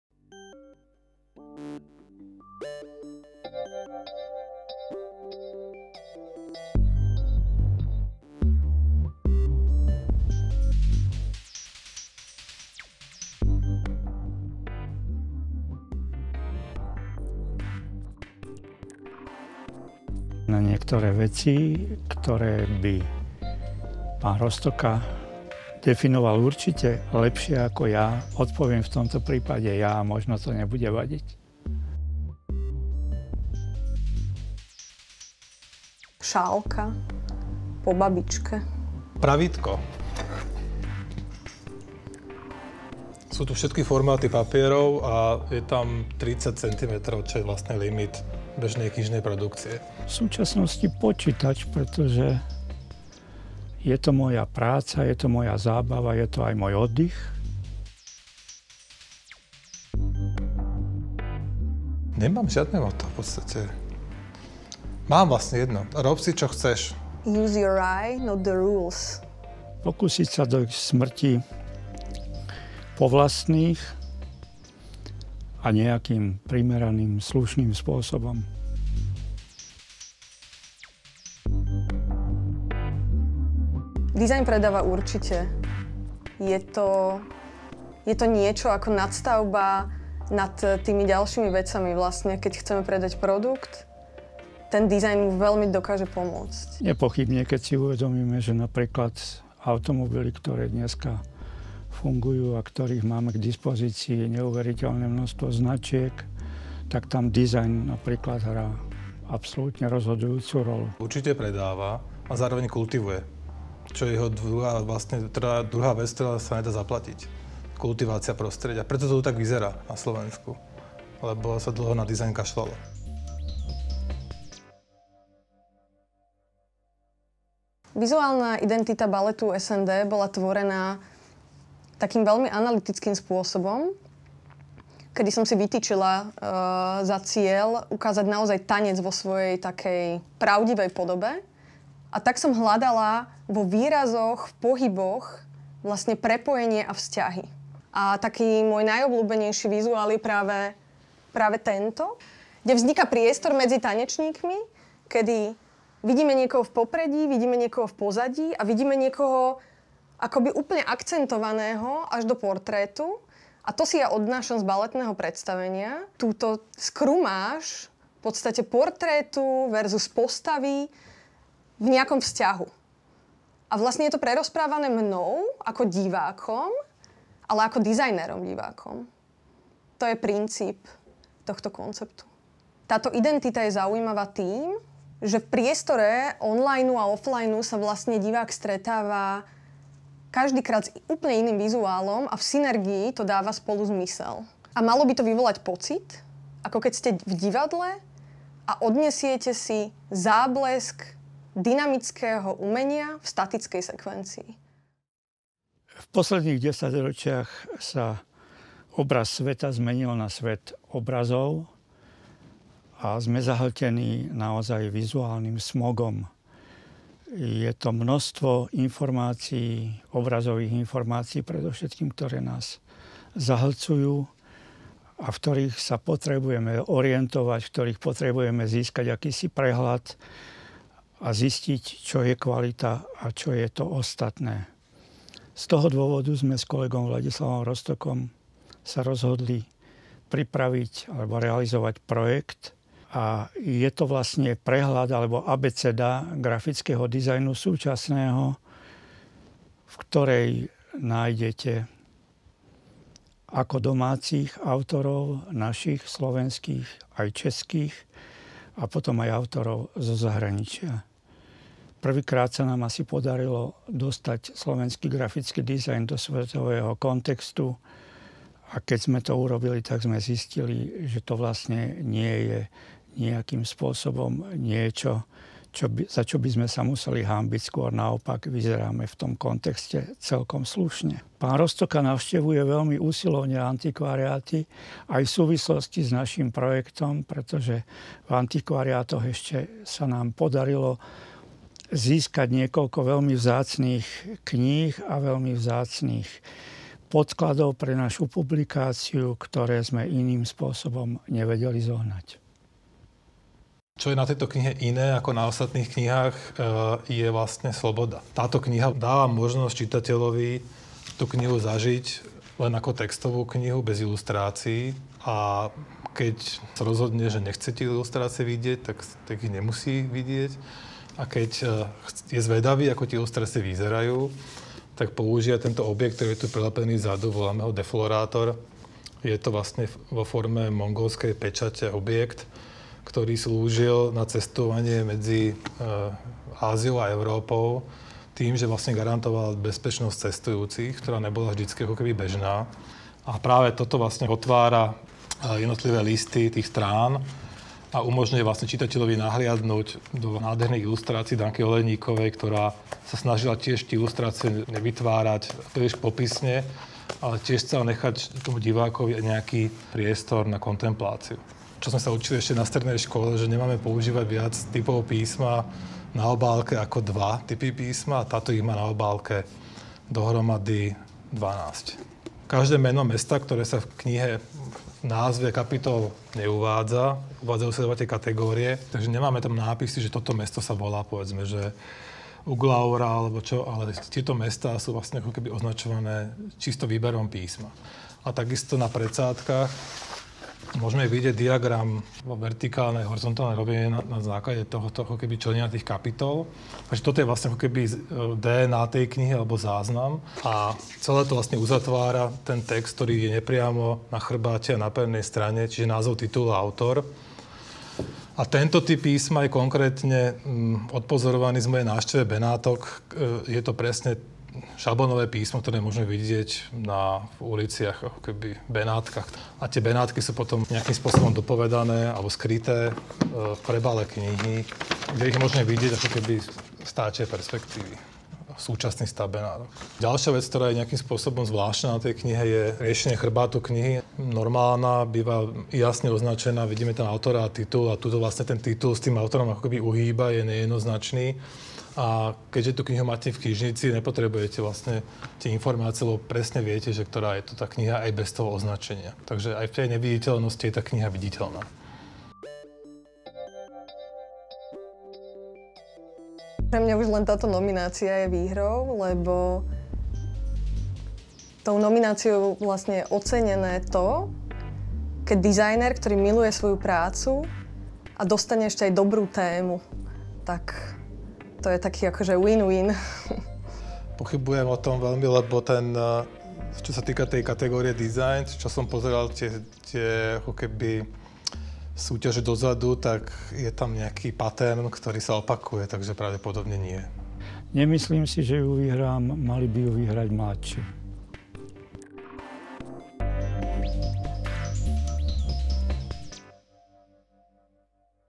Nominácie v Hlavnej cene kategórie Dizajn
Odborná  porota  zverejnila  mená  nominovaných  umelcov  Ceny  Nadácie  Tatra banky  za  umenie  2017  v  šiestich  oceňovaných  kategóriách.
Predstavujeme vám autorov a ich diela z kategórie Dizajn.

Pavol Bálik, Mikina Dimunová, Dušan Junek a Vladislav Rostoka.

Autor fotografií nominovaných: Peter Frolo
Autor fotografií ku kampani Zaži skutočnú vášeň pre Balet SND: Peter Brenkus